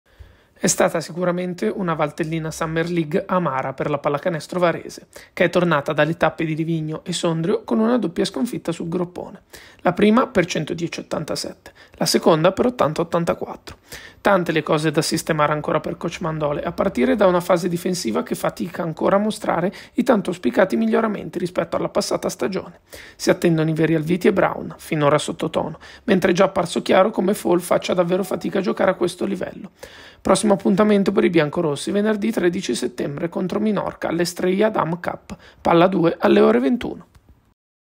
Basket - Varese riflette dopo le sconfitte nelle amichevoli
E' stata sicuramente una Valtellina Summer League amara per la Pallacanestro Varese che è tornata dalle tappe di Livigno e Sondrio con una doppia sconfitta sul groppone: la prima per 110-87, la seconda per 80-84. Tante le cose da sistemare ancora per coach Mandole, a partire da una fase difensiva che fatica ancora a mostrare i tanto auspicati miglioramenti rispetto alla passata stagione. Si attendono i veri Alviti e Brown, finora sottotono, mentre è già apparso chiaro come Fall faccia davvero fatica a giocare a questo livello. Prossimo appuntamento per i biancorossi, venerdì 13 settembre contro Minorca all’Estrella Damm Cup, palla a due alle ore 21:00.